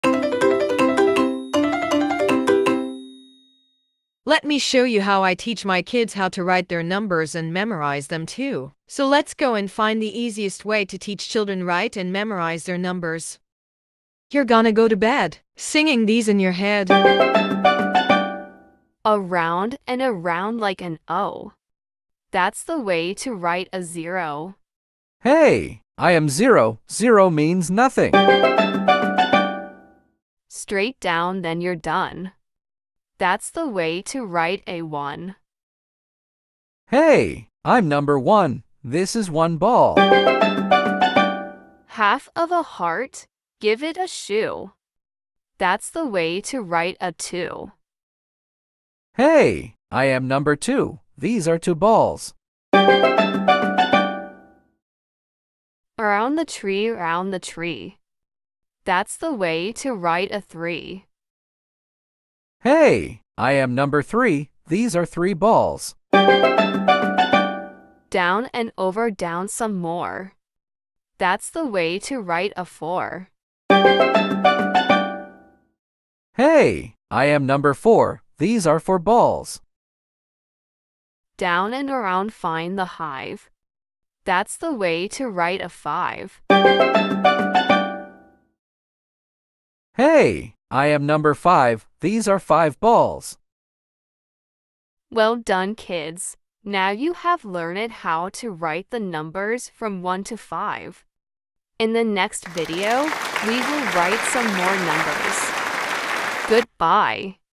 Writing Numbers from 0 to 5 For Children | Count and write Numbers| Write zero to five| Part 2
Hi Kids, Welcome to LEARNING ADVENTURE-TV!!
After learning about Letters in the previous video, Now you will learn how to write Numbers from 0 to 5  and also count the balls .